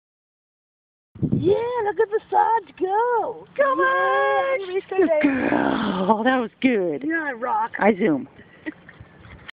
Zooming Marge
Marge showing off how well her new knees are working. She zooms her little butt off.